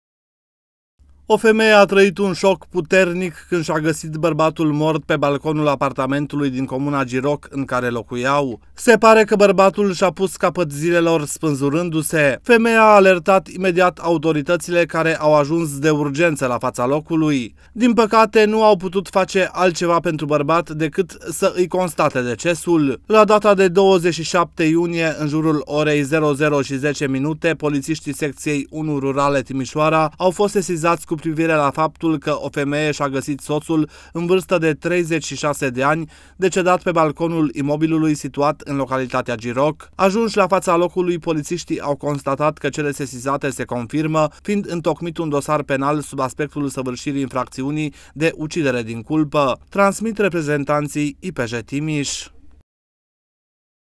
DRAMĂ LA GIROC! ȘI-A GĂSIT SOȚUL SPÂNZURAT
O femeie a trăit un șoc când și-a găsit bărbatul mort pe balconul apartamentului din comuna Giroc în care locuiau. Se pare că bărbatul și-a pus capăt zilelor spânzurându-se. Femeia a alertat imediat autoritățile care au ajuns de urgență la fața locului. Din păcate, nu au putut face altceva decât să constate decesul bărbatului. ”La data de 27 iunie 2024, în jurul orei 00:10, polițiștii Secției 1 Rurale Timișoara au fost sesizați cu privire la faptul că o femeie și-a găsit soțul, în vărstă de 36 de ani, decedat pe balconul imobilului situat în localitatea Giroc